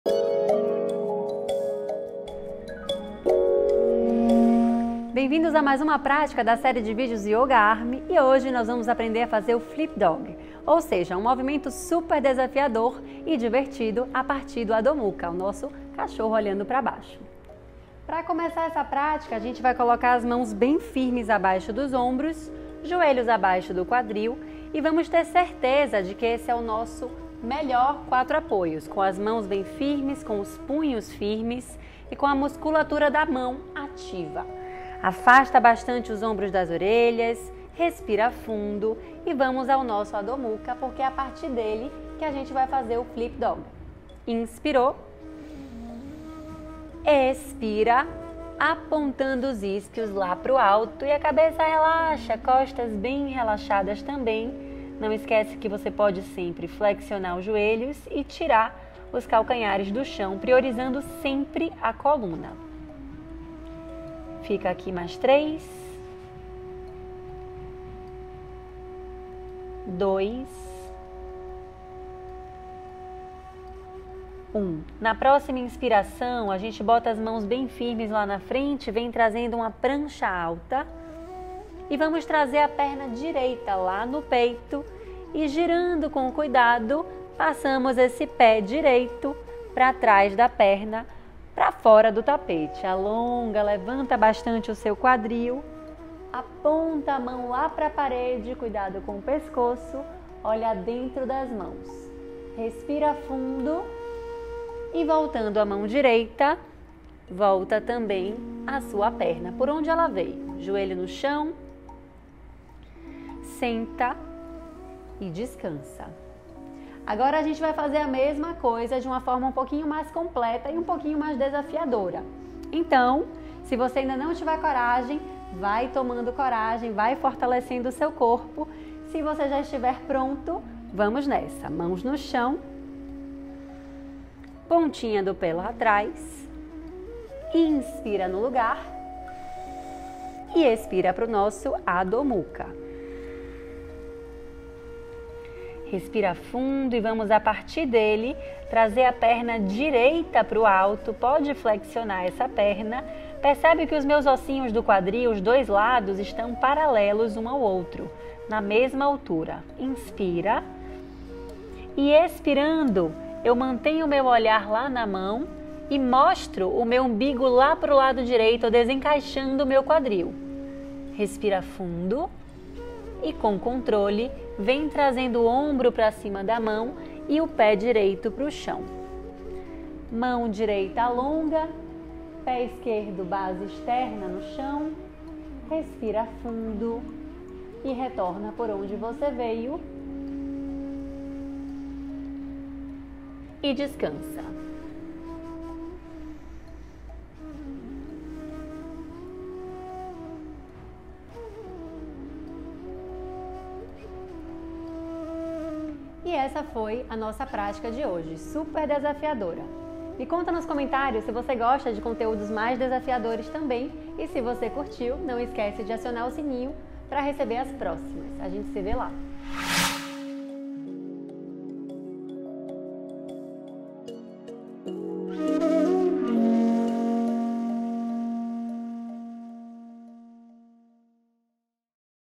YOGA para FORTALECER os MÚSCULOS: aprenda a pose de CAMATKARASANA [YOGA ARMY]
Você está em busca de fortalecimento muscular e um corpo tonificado? Neste vídeo vamos explorar a poderosa pose de yoga chamada Camatkarasana, também conhecida como Flip Dog ou Wild Thing Pose.

Essa é uma pose intermediária que desafia seus músculos e proporciona um alongamento profundo, aumentando a flexibilidade da coluna vertebral e ajudando a fortalecer o core, os ombros, o peito e os braços.

Neste tutorial, você terá o passo a passo da sequência de movimentos necessários para entrar e sair da Camatkarasana de forma segura e eficaz. Pratique regularmente e sinta seu corpo ficar mais forte, flexível e equilibrado.

00:00 Boas-vindas
00:21 Posição Camatkarasana
03:19 Despedida

Gostou da prática? Já comenta aqui embaixo como foi a sua experiência.